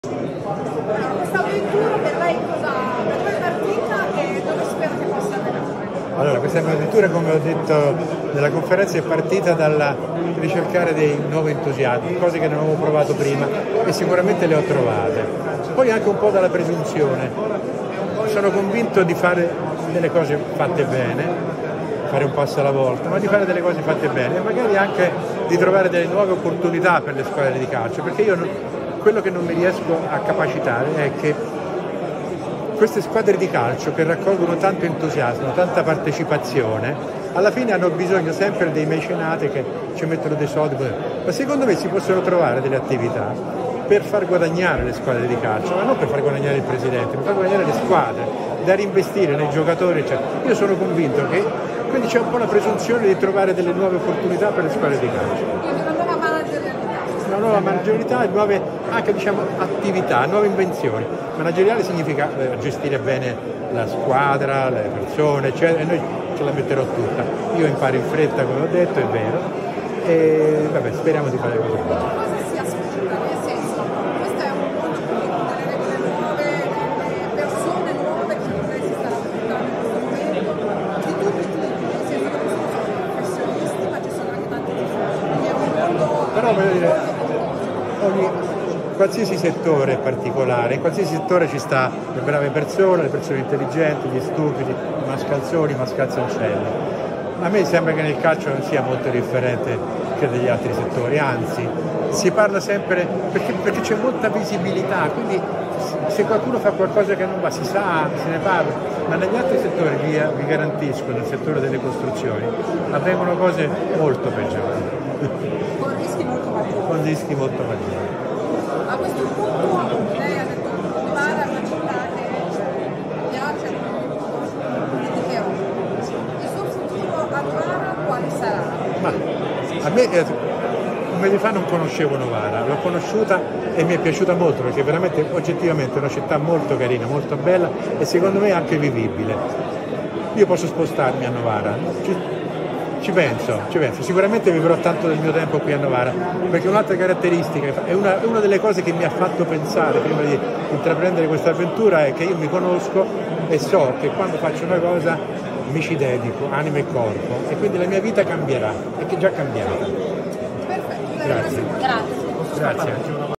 Conferenza Novara Football Club 03 09 2021 Massimo Ferranti dichiarazioni
Dichiarazioni post-conferenza stampa presso il Castello di Novara